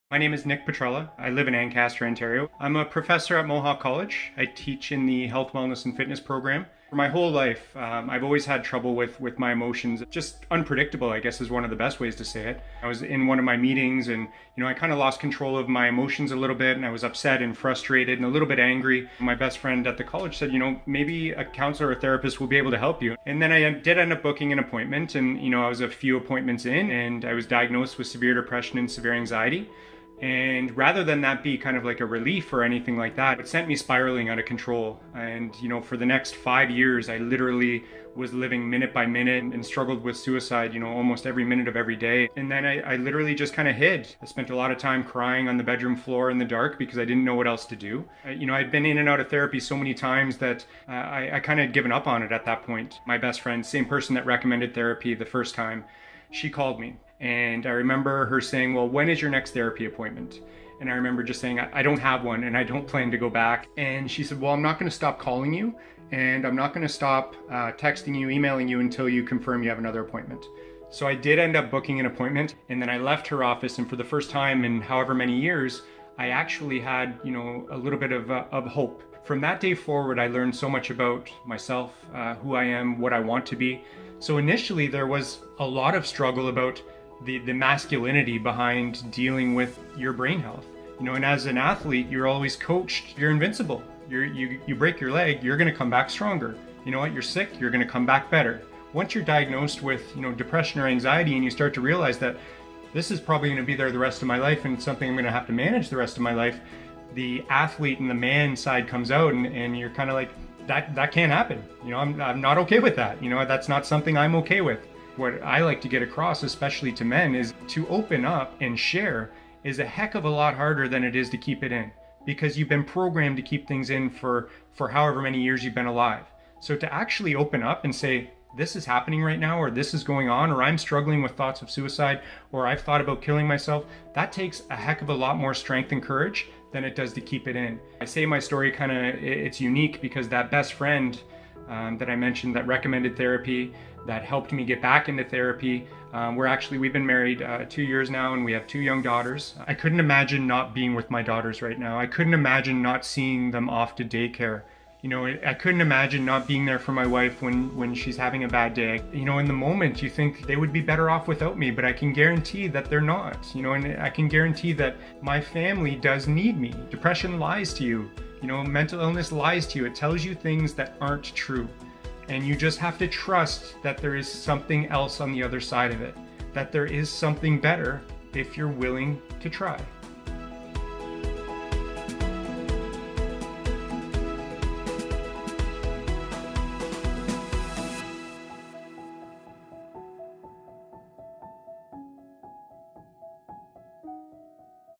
Nick Petrella - 2020 Face of Mental Illness
Diagnosed with severe depression and anxiety in 2010, Nick initially felt shame and guilt. Over the next five years he saw a dozen different mental health professionals. He finally found a therapist who was able to help him and slowly began his recovery. With the support of his wife, his therapist, his friend at CMHA, his two young daughters and three dogs, Nick lives and breathes mental health awareness to reduce stigma and prove that recovery is possible every day. He proudly educates others and is the co-founder of Mental Health in Motion at Mohawk College where he is a professor.